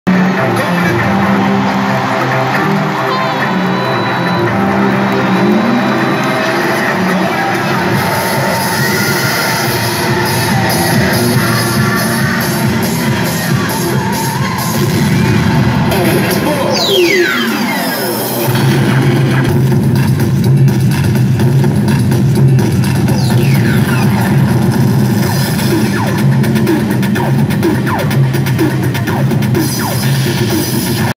Game 2 UAAP 81 FINALS: UP VS ADMU
Araneta Coliseum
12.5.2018